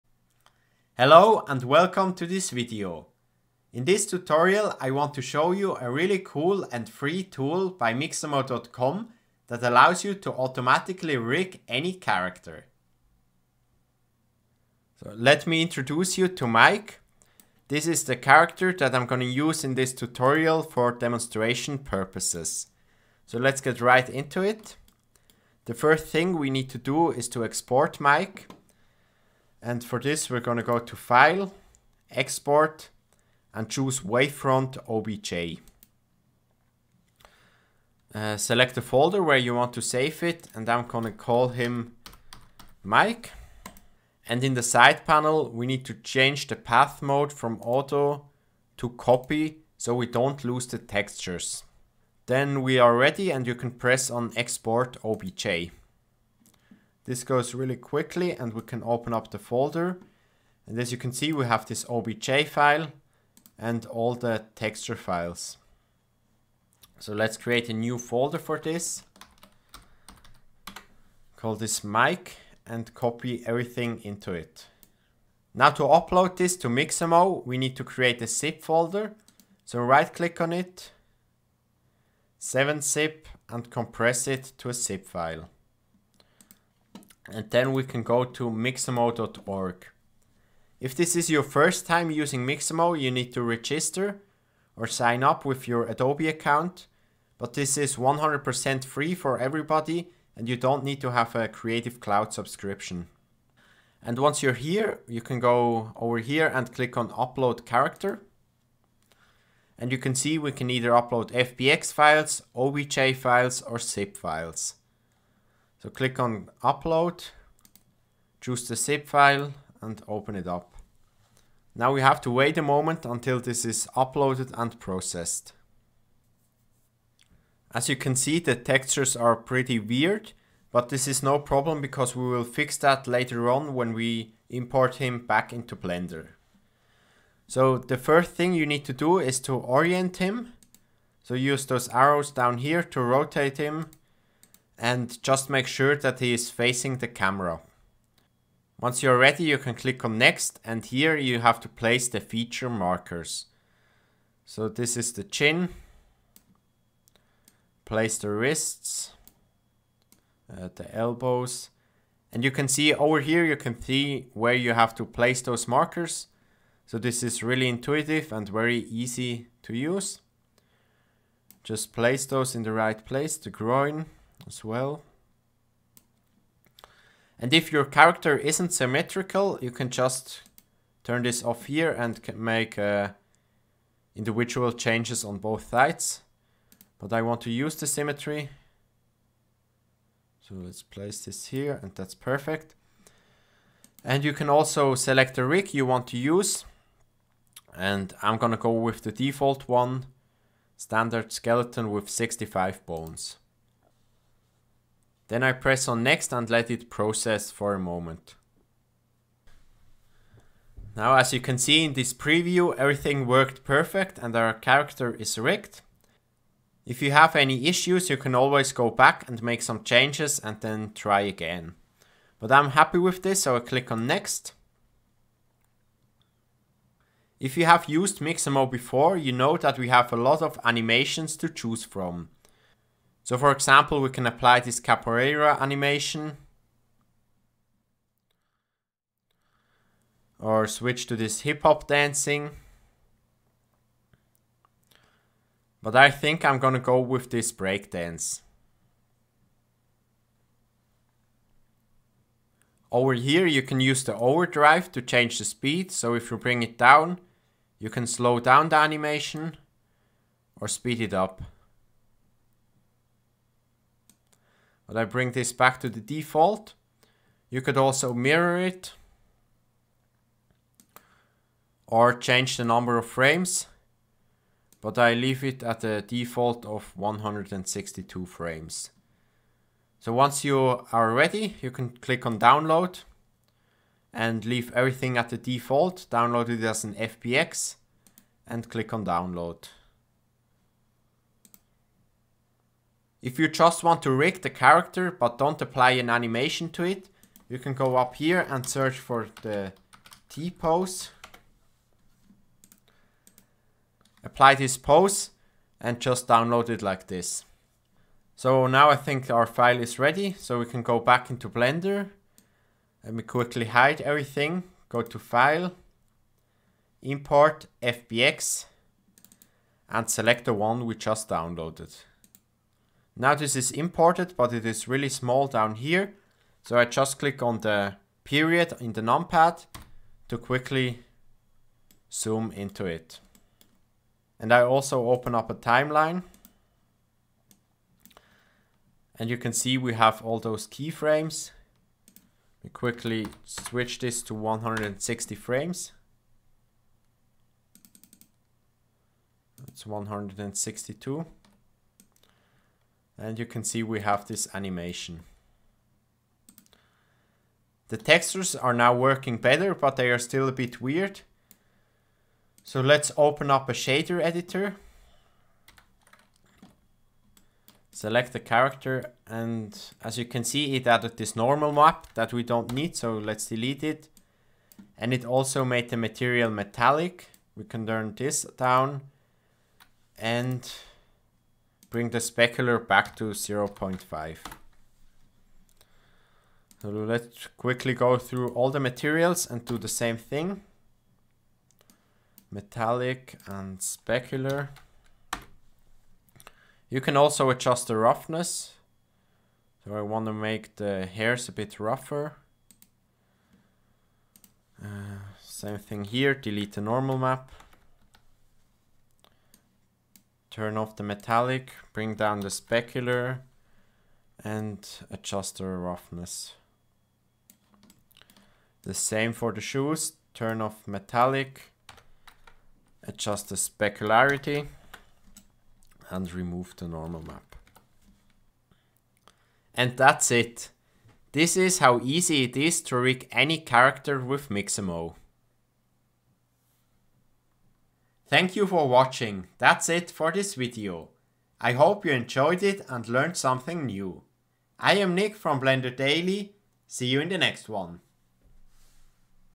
Rig any Character with the Mixamo Auto-Rigger!
In this video I want to show you how to use the Mixamo auto rigging tool, to quickly rig and animate any 3d character. I also demonstrate how to bring it back into Blender and fix texture issues.